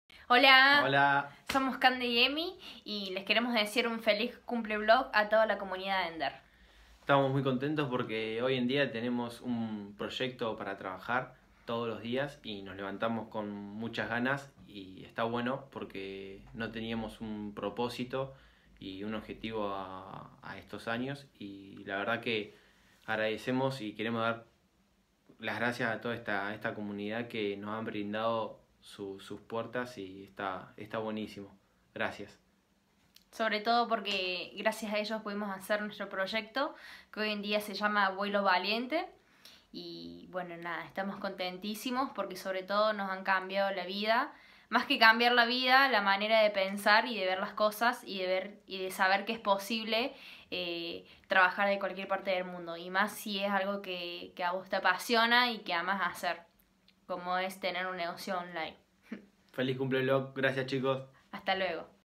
Candy y Emi - Escuela Nómada Digital e Inteligencia Viajera
En el año 2019 me puse un reto personal y profesional. Quería cambiar la vida a 50 personas, y esto es lo que pasó... ¡56 vidas cambiadas!

Si quieres saber más sobre ellos y sobre mi 2019, no te pierdas este post

- Marta y Noelia, alumnas de la END.

- Raúl Cánovas, alumno de la END.

- Albert Aguilar de padresrebeldes.com, alumno y tutor de la END.

- Juan y Rocío de parateavivir.com.

- Vicky, alumna de la END.

- Toni Durán, alumno de la END.

- Fabiola Fernández de @beatbuildschool

- Luis Manuel, alumno de la END.

- Elvira Castro de yogaenfamiliaonline.com.

- Tony Rodríguezcolitisenpareja.comviajarapedales.com

- Diego Azúacolitisenpareja.commejoracorriendo.com

- Alicia Vasii, alumna de la END.

- Yvonne, alumna de la END.

- Pablo Domínguez, alumno de la END.

- Tania Pina de Madre sin estrés

- Amaya Andrés

- Rafael Díaz-Jargüin Hierro

- Margie Carrillo de @margiefer

- Roberto Bustamante

- Sofia Mateo de pequeñasdosis.com